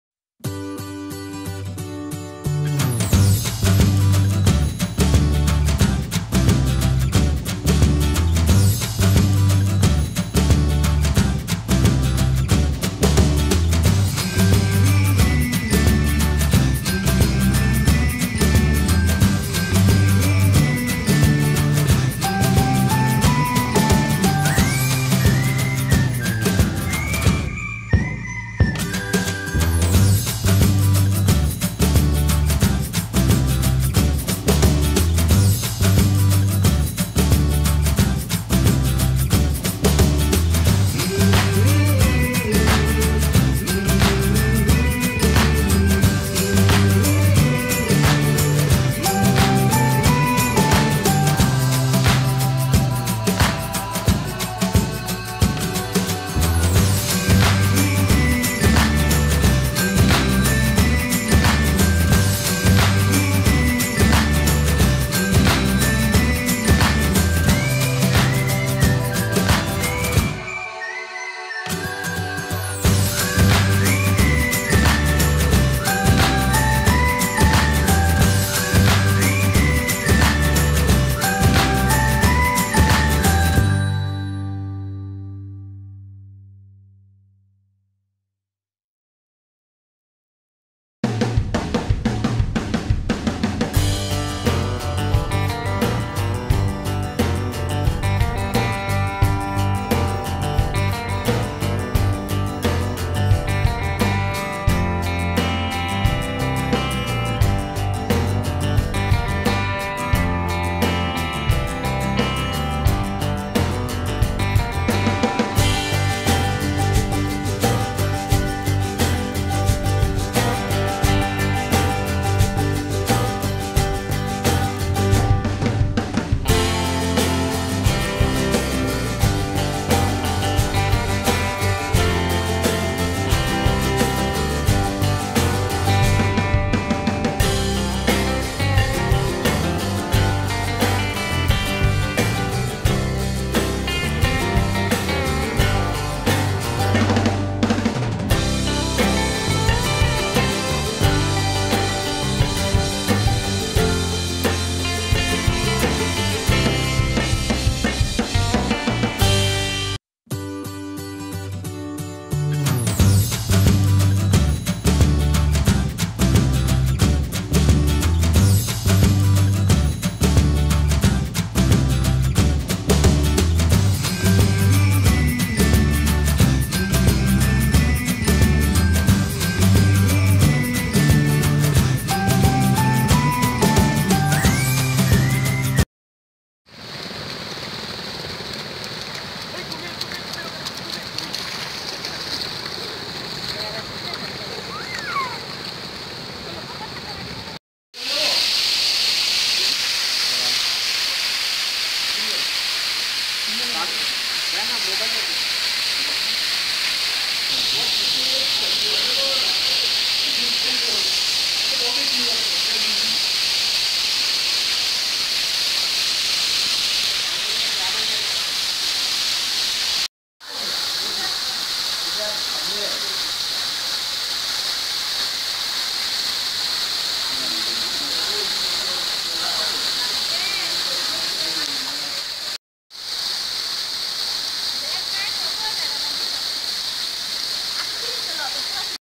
Cox'sbazar | Himchori Waterfall | Inani Beach | The Largest Sea Beach in the World
When I visited cox'sbazar, himchori waterfall & coral beach I captured this video.Hope all of you'll enjoy it.